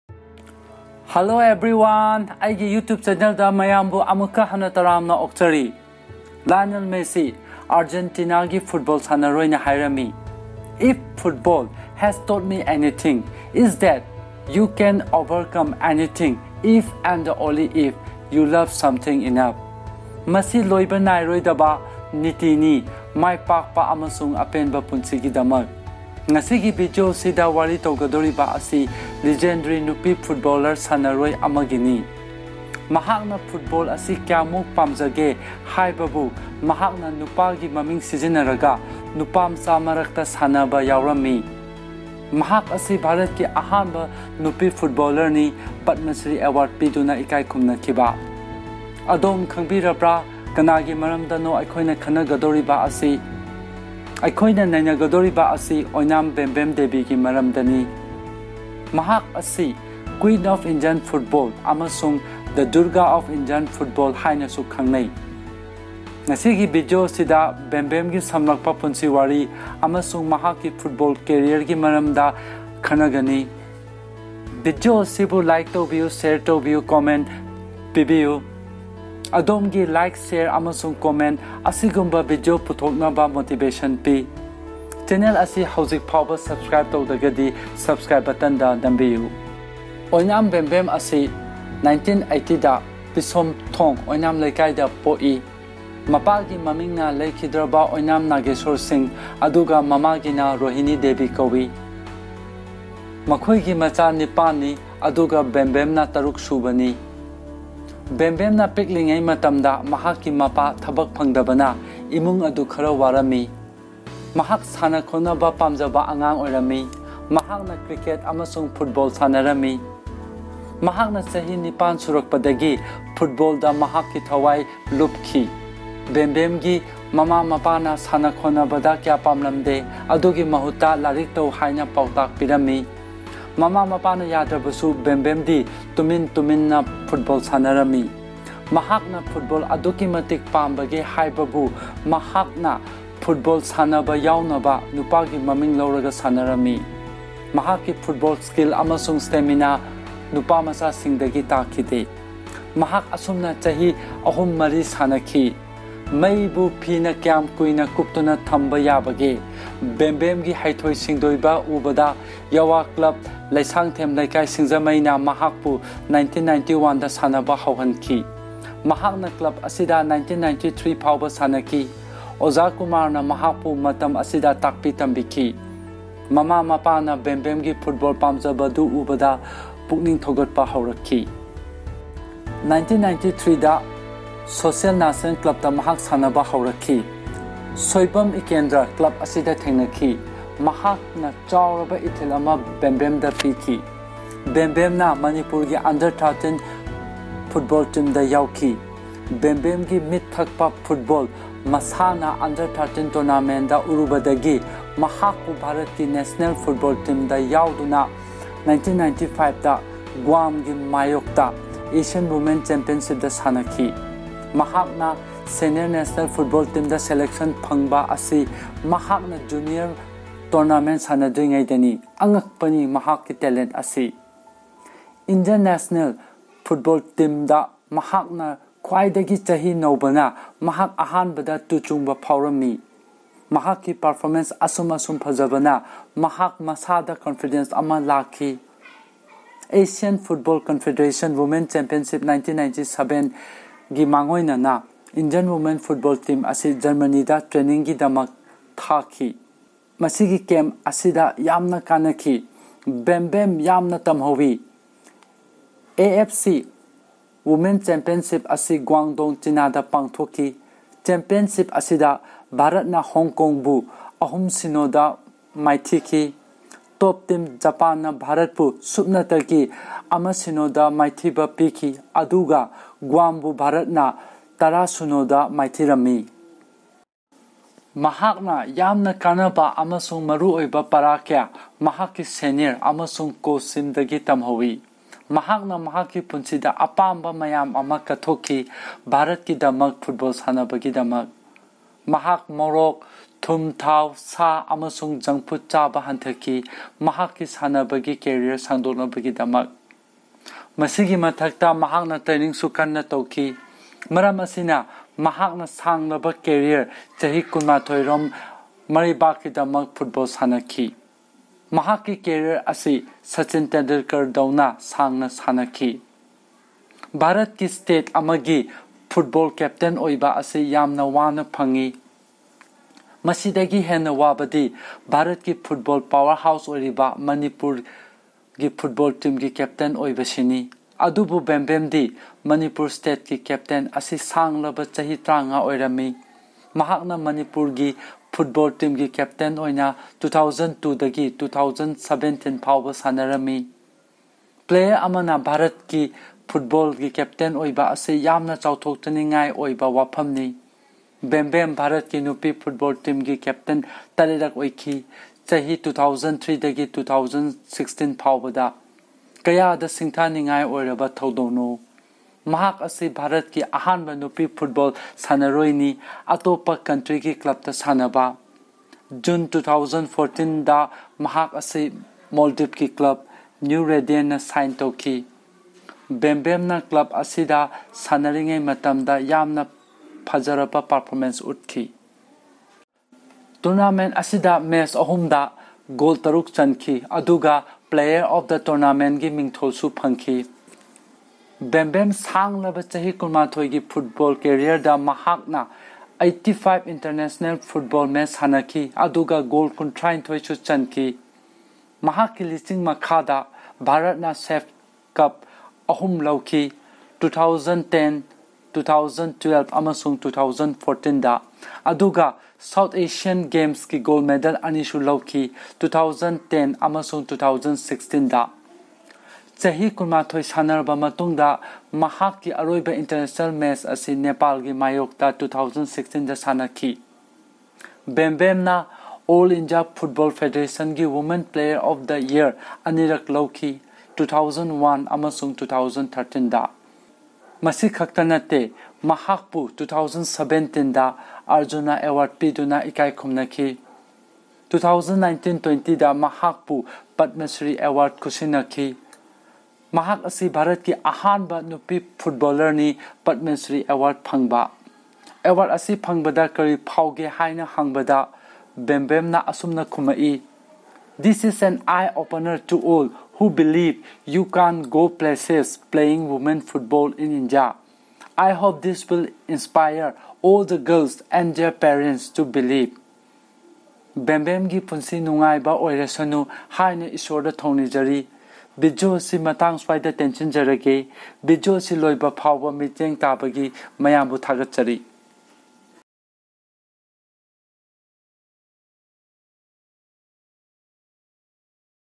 Oinam Bembem Devi: The Durga of Indian Women Football Team (Manipuri)
This is a Manipuri video on the Durga of the Indian Women Football team- Oinam Bembem. This is the life story of the legendary football player of India. Enjoy the video.